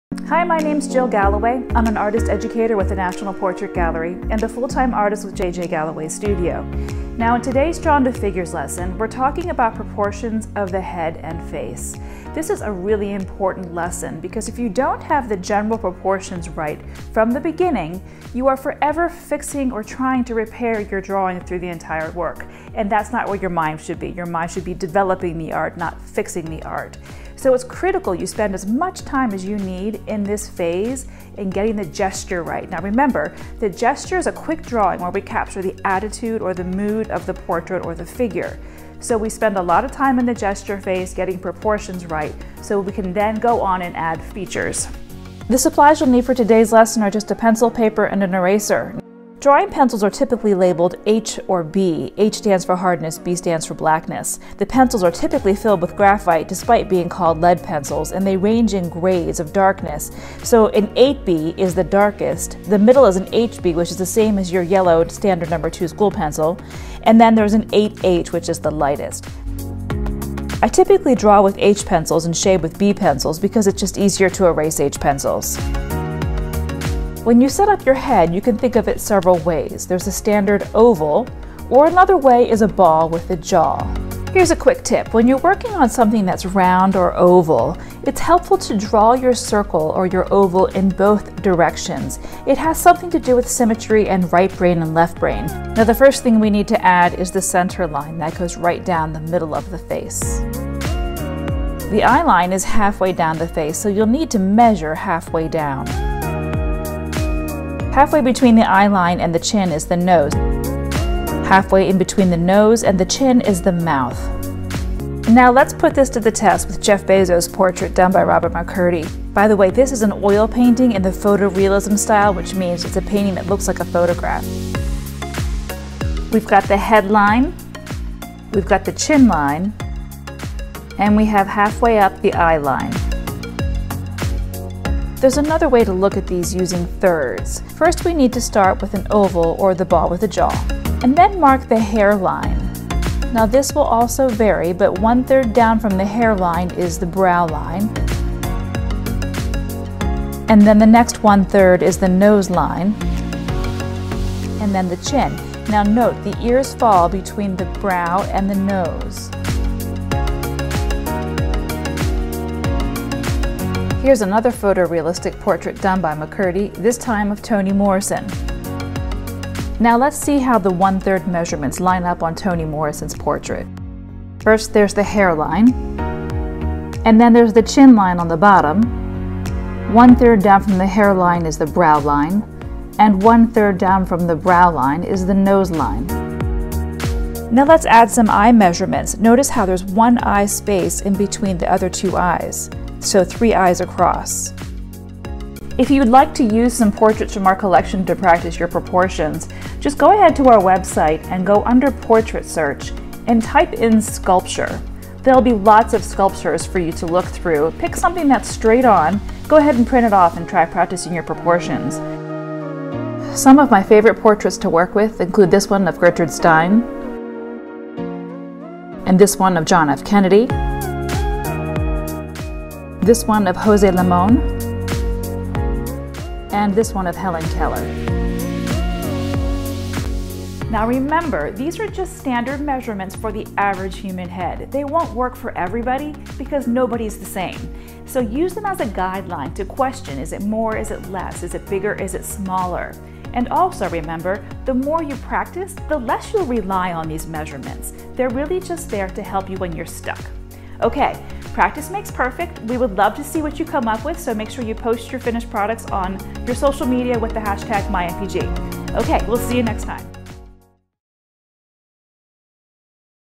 Drawn to Figures: Proportions of the Head
Learn basic tips and steps on sketching the head for your figure drawings.